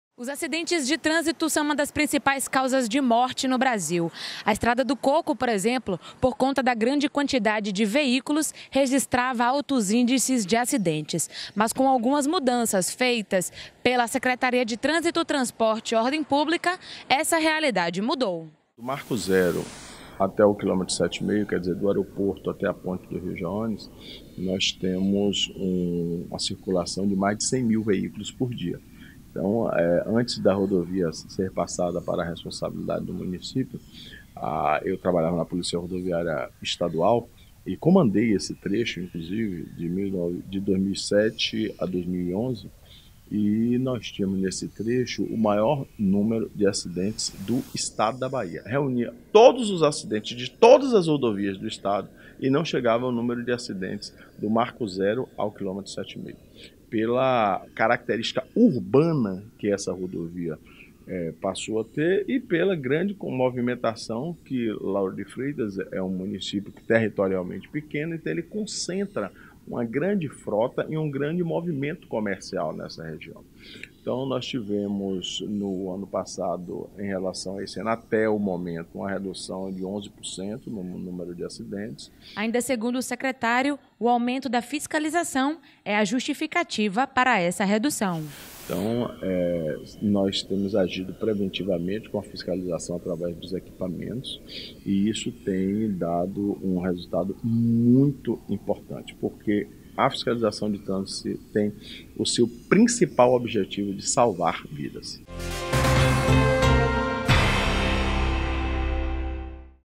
Dados Acidentes 2019
No último ano, ações de fiscalização reduziram o índice de acidentes em Lauro de Freitas. Confira: